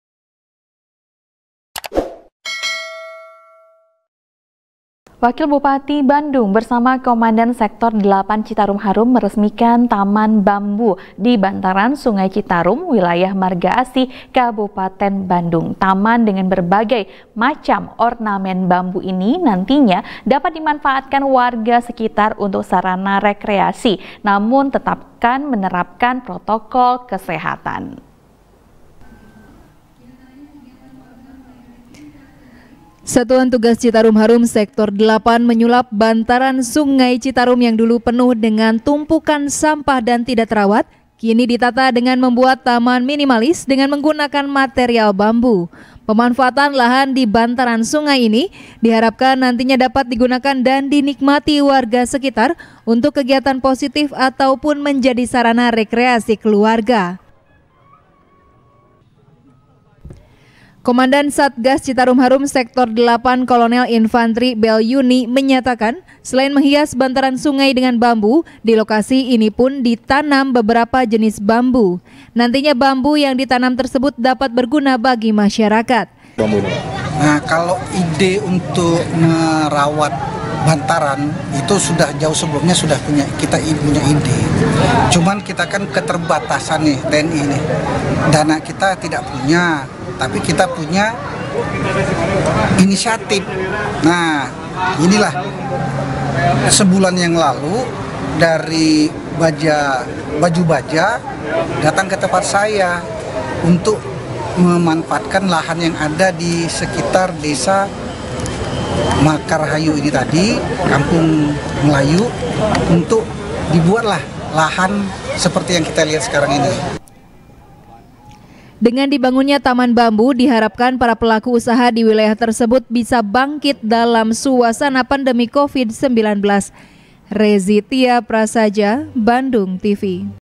Dimasa Pandemi, Satgas Citarum Harum Dirikan Taman Bambu | SBR | BANDUNGTV
SBR | BANDUNGTV | KRISANTI | Wakil Bupati Bandung, Bersama Komandan Sektor 8 Citarum Harum, Meresmikan Taman Bambu, Di Bantaran Sungai Citarum, Wilayah Margaasih, Kabupaten Bandung. Taman Dengan Berbagai Macam Ornamen Bambu Ini, Nantinya Dapat Di Manfaatkan Warga Sekitar Untuk Sarana Rekreasi, Namun Tetapkan Menerapkan Protokol Kesehatan.